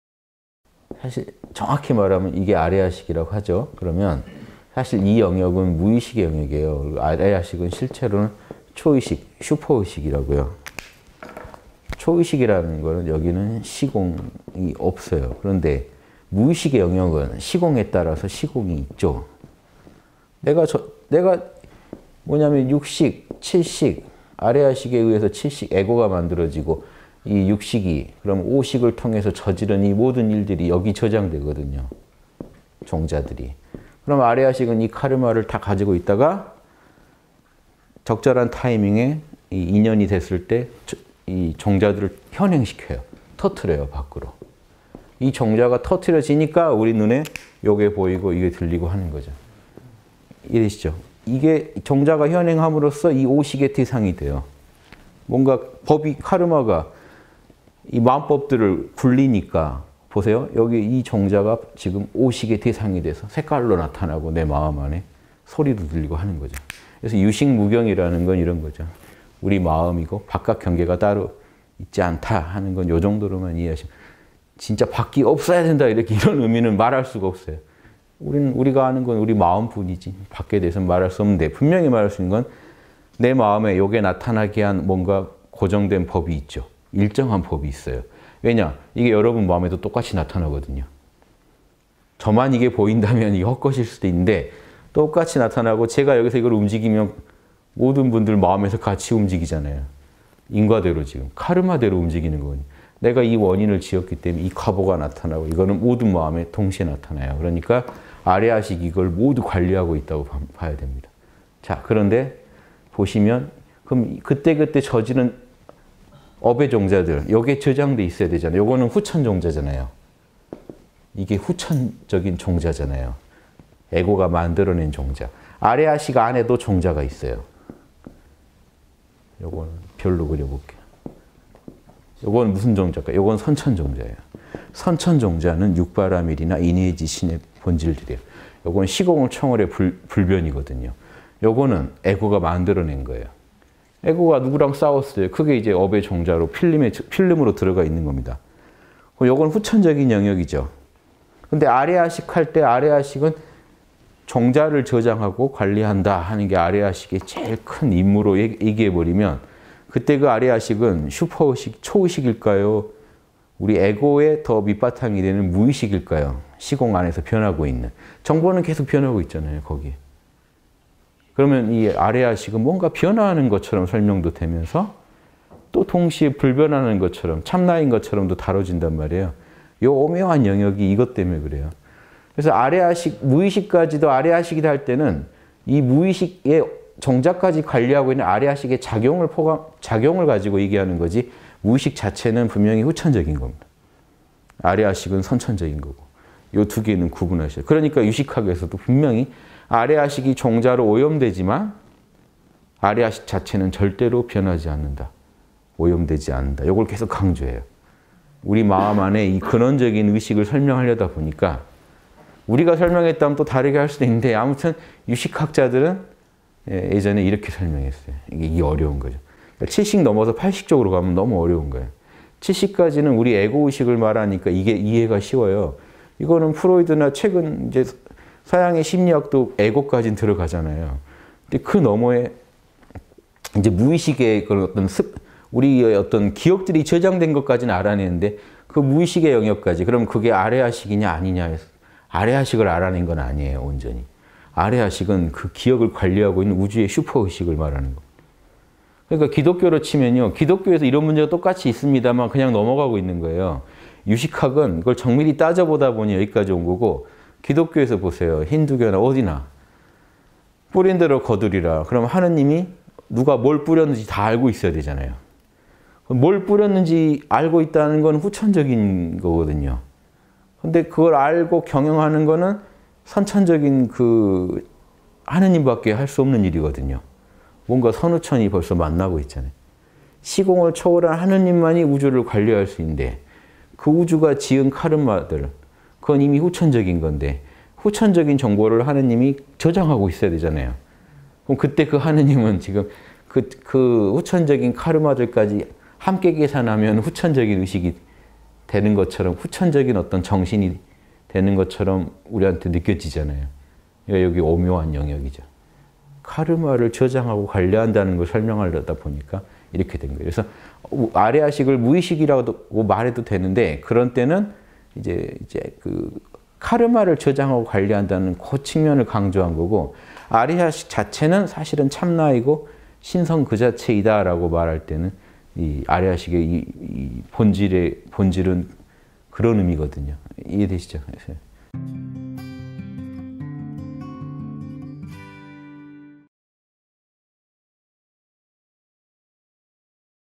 [3분 법문] 아뢰야식 설명 _홍익선원.윤홍식.Z69
아뢰야식의 실체는 초의식, 슈퍼의식이에요.
초의식이라는 건, 여기에는 시공이 없어요.
그런데 무의식의 영역은 시공이 있죠.

뭐냐하면 6식, 7식.
아뢰야식에 의해서 7식(에고)가 만들어지고
이 6식, 즉 5식을 통해서 저지른 모든 일들이
여기(무의식) 저장 되거든요, 종자들이.
그러면 아뢰야식은 이 카르마를 다 가지고 있다가
적절한 타이밍에 인연이 됐을 때 종자들을 현행시켜요.
밖으로 터트려요.
이 종자가 터트려지니까 우리 눈에 이게 보이고, 이게 들리고 하는 거죠.
이해되시죠?

이게, 종자가 현행함으로서 이 5식의 대상이 돼요.
뭔가 법이, 카르마가 이 만법들을 굴리니까.
여기 이 종자가 지금 5식의 대상이 돼서 내 마음 안에 색깔로 나타나고,
소리도 들리고 하는 거죠.

그래서 '유식무경唯識無境'이라는 건 이런 거죠.
'우리 마음이고 바깥 경계가 따로 있지 않다.'
하는 건 이 정도로만 이해하시면 됩니다.
'진짜 밖이 없어야 한다.' 이렇게는 말할 수가 없어요.
우리가 아는 건 우리 마음뿐이지 밖에 대해서는 말할 수 없는데,
분명히 말할 수 있는 건, 내 마음에 이게 나타나게 한 뭔가
고정된 법이 있다는 정도죠.일정한 법이 있어요.

왜냐, 이게 여러분 마음에도 똑같이 나타나거든요.
저만 이게 보인다면 이게 헛것일 수도 있는데 (모두에게) 똑같이 나타나고,
제가 여기서 이걸 움직이면 모든 분들 마음에서 같이 움직이잖아요.
인과대로 지금, 카르마대로 움직이는 거거든요.
내가 이 원인을 지었기 때문에 이 과보가 나타나고,
이건 모든 마음에 동시에 나타나요.
그러니까 아뢰야식이 이걸 모두 관리하고 있다고 봐야 합니다.

그런데 아뢰야식을 말할 때
아뢰야식은 '종자를 저장하고 관리한다.' 하는 게
아뢰야식의 제일 큰 임무로 얘기해 버리면,
그때 그 아뢰야식은 슈퍼의식, 초의식일까요?
우리 에고의 더 밑바탕이 되는 시공 안에서 변하고 있는 무의식일까요?
정보는 계속 변하고 있잖아요, 거기에는.

그러면 이 아뢰야식은 뭔가 변화하는 것처럼 설명도 되면서,
또 동시에 불변하는 것처럼, 참나인 것처럼도 다뤄진단 말이에요.
이 오묘한 영역이 이것 때문에 그래요.
그래서 "무의식까지도 아뢰야식이다."라고 할 때는 이 무의식의 종자까지
관리하고 있는 아뢰야식의 작용을 가지고 얘기하는 거지,
무의식 자체는 분명히 후천적인 겁니다.
아뢰야식은 선천적인 거고.
이 두 개는 구분하셔야 합니다.

-본 동영상의 외국어 번역 및 녹취작업 등 재능기부를 받습니다.

* 홍익학당 후원하기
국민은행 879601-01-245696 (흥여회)